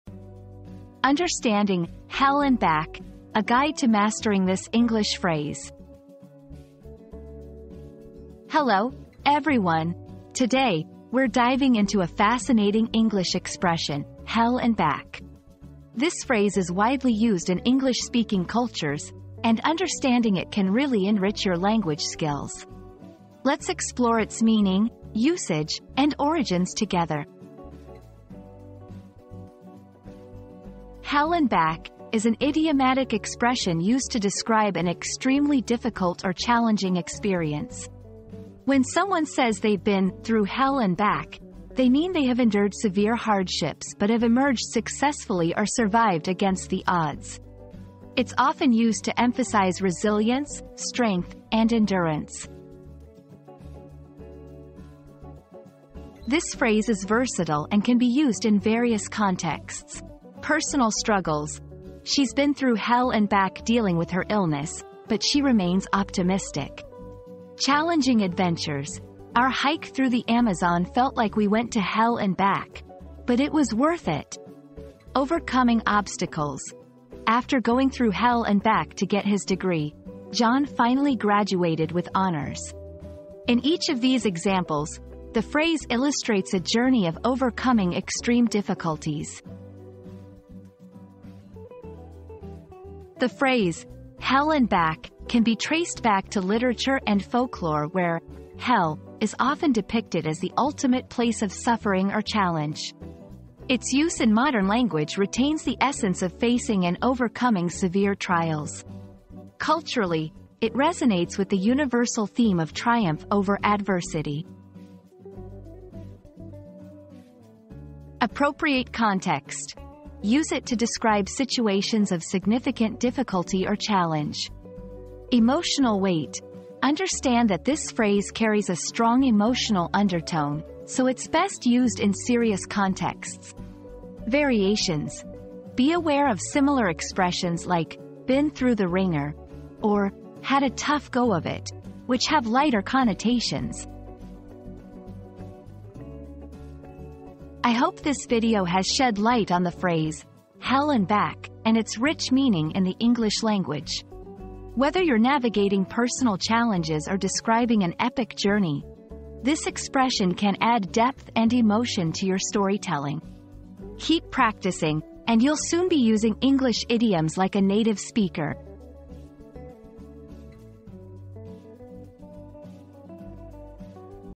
Understanding "Hell and Back": A Guide to Mastering This English Phrase
Mastering 'Hell and Back': Your Guide to Understanding This English Phrase • Unlock the secrets of the popular English phrase 'Hell and Back' with this comprehensive guide. Learn its origin, usage, and different interpretations, and gain confidence in incorporating it into your everyday conversations. Join us on this linguistic journey and conquer 'Hell and Back' like a pro!

00:00 • Introduction - Understanding "Hell and Back": A Guide to Mastering This English Phrase
00:30 • The Meaning of "Hell and Back"
00:55 • Usage in Context
01:32 • Historical and Cultural References
01:57 • Tips for Using "Hell and Back"